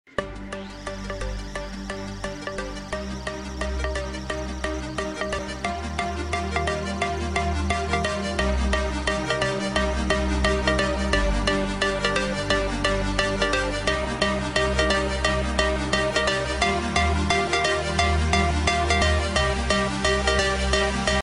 GAV MCLAREN P1
It has an 8 Cylinder engine plus an Electric Motor, Producing a total of 1500HP. Topped out at 155MPH according to testing. It also has a working dynamic wing.